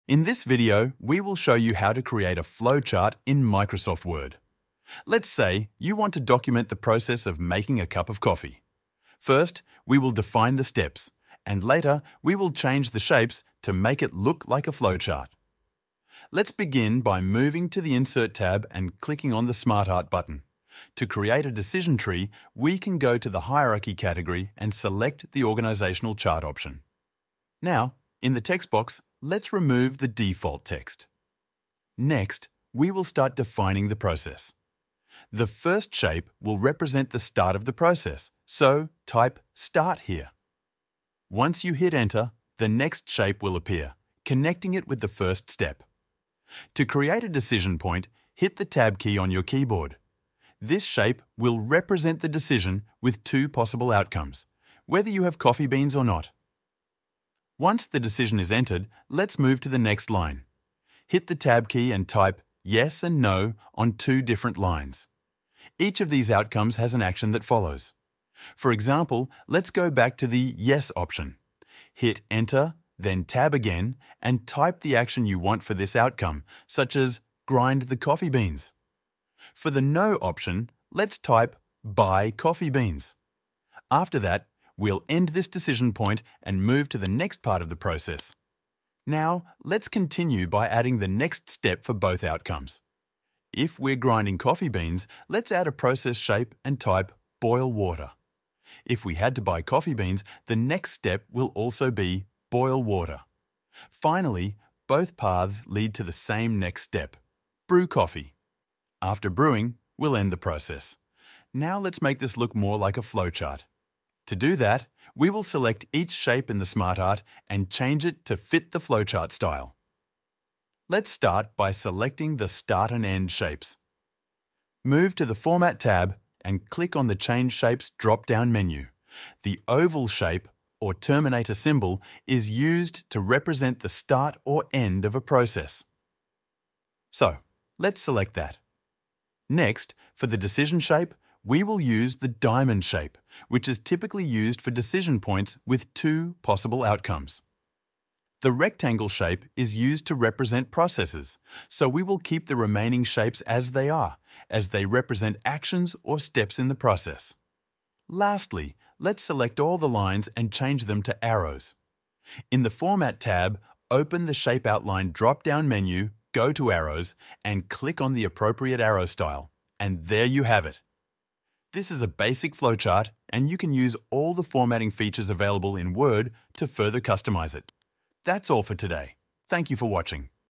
How to Make a Flowchart with SmartArt in Word
Want to document a process in a clean, visual way—without leaving Microsoft Word? This tutorial shows you how to create a simple, clear, and useful flowchart using Word's built-in SmartArt tools. Whether you're preparing a report, planning a workflow, or explaining a procedure, this guide helps you get it done fast.

🔹 What You’ll Learn in This Video:
✅ How to access and use SmartArt in Microsoft Word
✅ How to build a process flow with decision points
✅ Tips for labeling shapes and adding logical flow
✅ How to format flowcharts using proper symbols
✅ Ways to customize shapes and connectors for clarity

⏱ Timestamps
00:00 – Introduction & Why Use a Flowchart
00:16 – Insert SmartArt and Select a Layout
00:35 – Define the Process Steps and Decisions
01:28 – Add Branches and Actions (Yes/No Paths)
02:00 – Format the Flowchart Using Correct Symbols

Flowcharts help you communicate complex processes in a simple way. If you're a student, educator, office worker, or content creator, this video helps you:
✔ Build professional visuals using tools you already have
✔ Avoid third-party software
✔ Save time while creating clean, structured diagrams
✔ Understand SmartArt features without reading lengthy manuals

👍 Like this tutorial? Help us grow the channel!